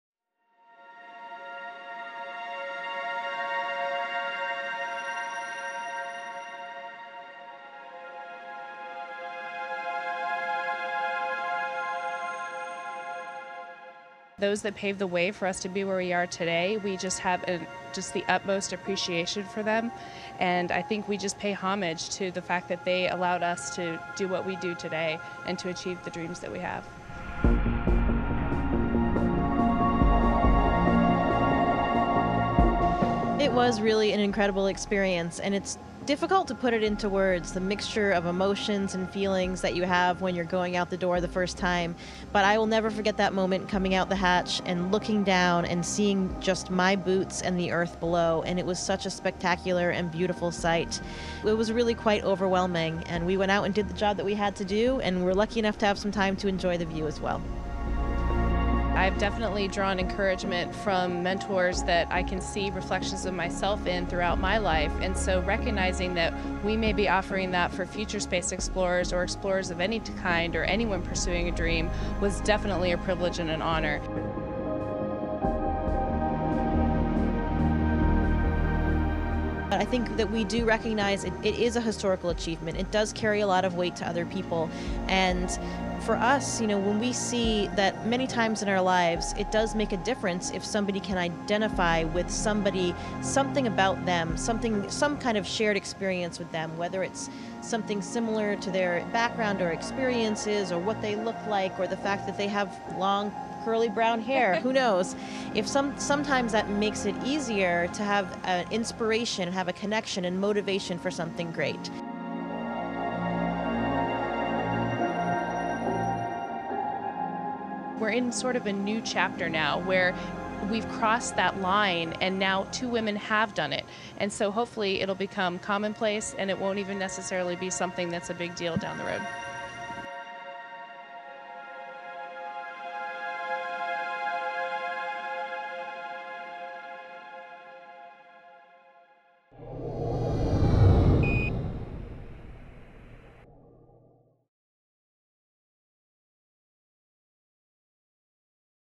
Astronauts Christina Koch and Jessica Meir reflect on the first All Woman Spacewalk MP4
NASA astronauts Christina Koch and Jessica Meir reflect on the historic spacewalk they performed on Oct. 18 – the first ever to be conducted by two women. They spent a total of 7 hours and 17 minutes in the vacuum of space. It was Koch’s fourth spacewalk and Meir’s first.